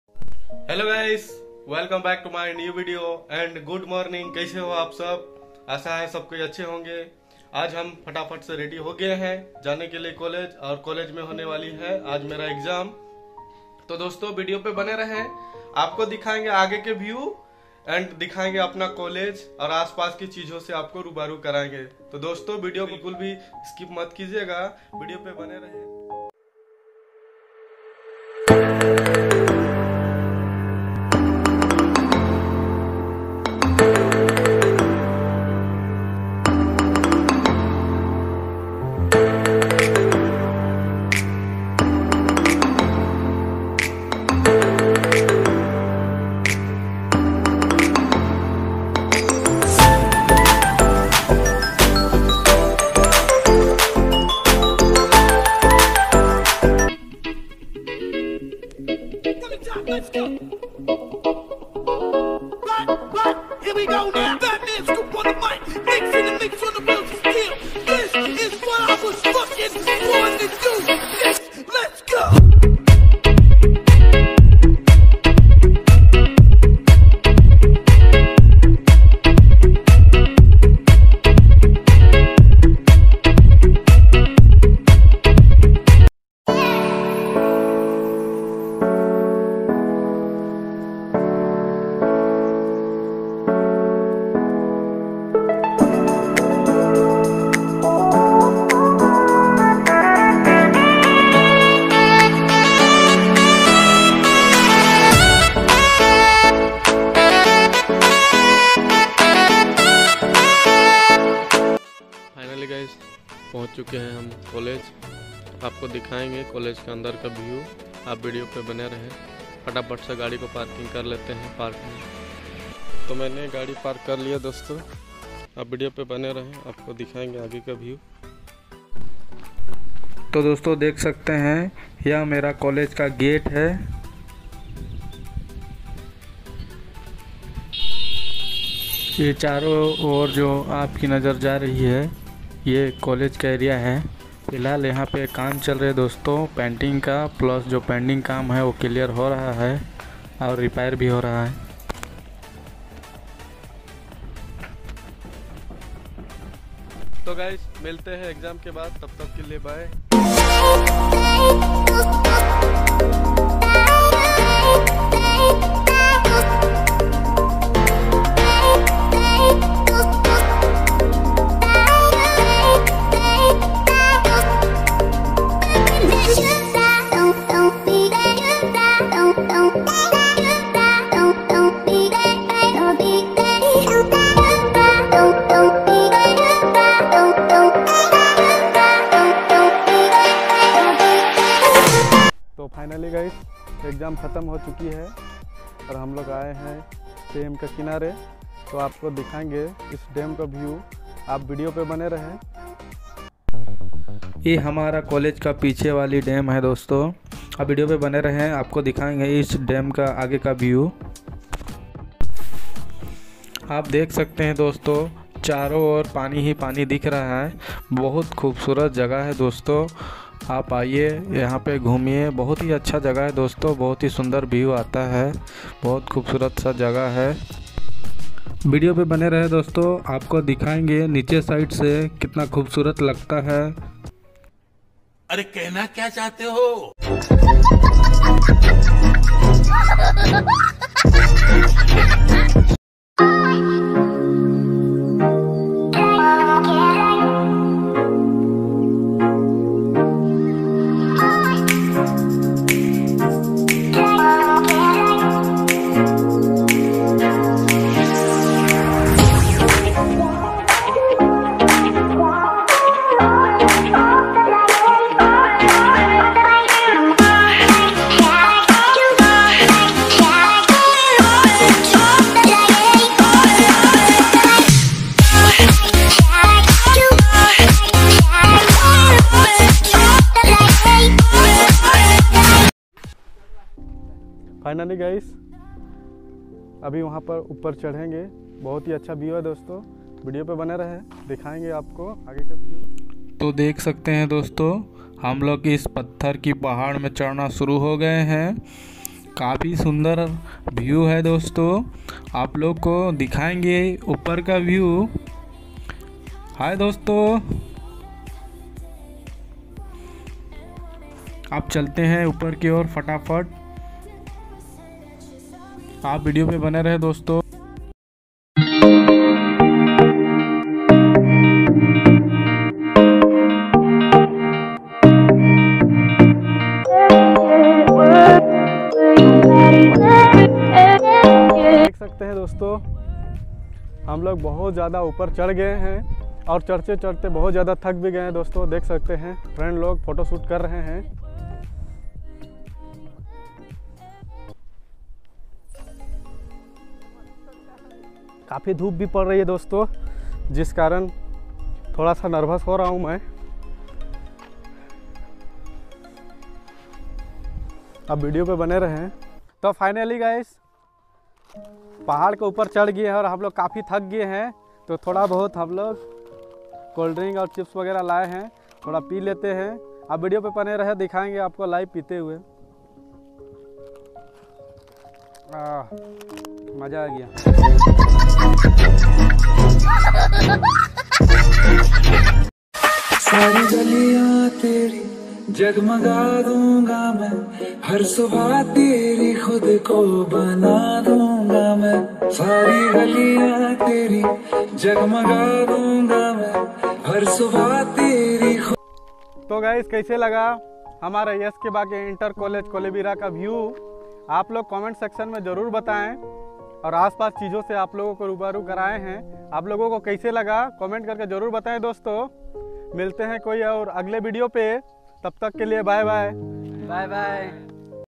S.K bage inter college kolebira Jharkhand//एस.के.बागे इन्टर कॉलेज कोलेबिरा @sureshvlog3969
Hello friends welcome to my new video Aaj ham es Video me Hamare College aur College ke as pas sunder aur beutiful area se jubaru karayenge aur subhi chijo ke bare complete jankari dunga
Aur apna friends ko share karen